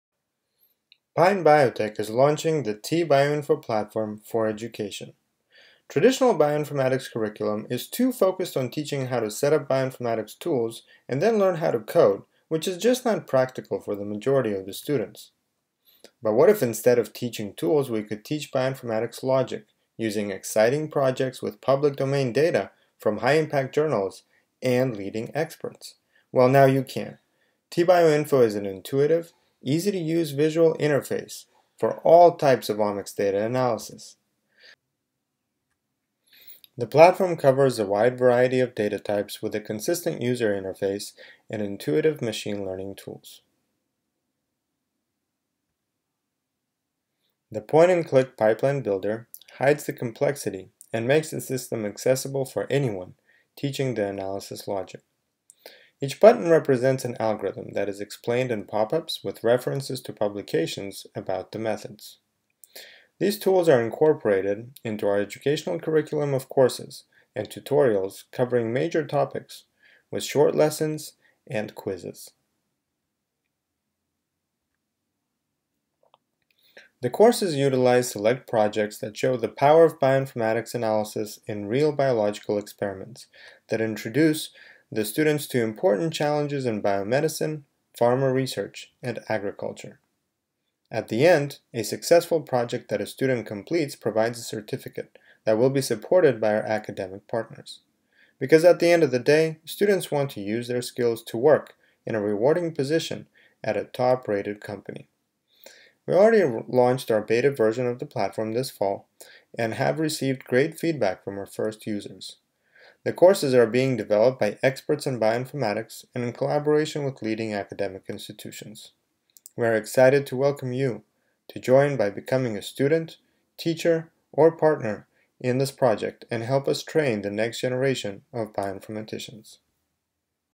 Bioinformatics education - let's start today!
BioInformatics education is now open for everyone - learn practical skills using interesting project data. Visual, interactive and accessible for anyone, even people that do not have any coding skills! Visit edu.t-bio.info to get started.